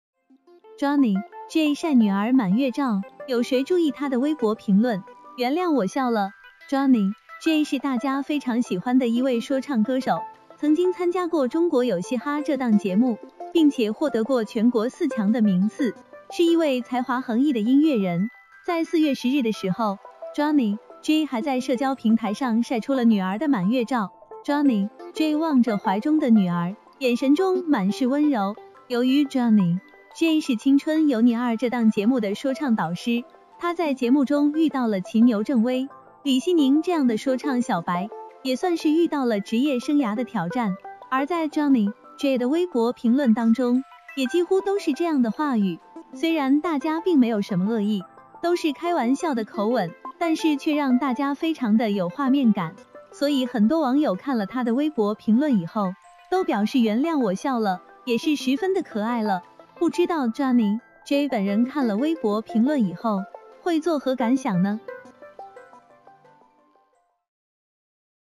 jony j晒女儿满月照,有谁注意他的微博评论？原谅我笑了
➤Thanks收看！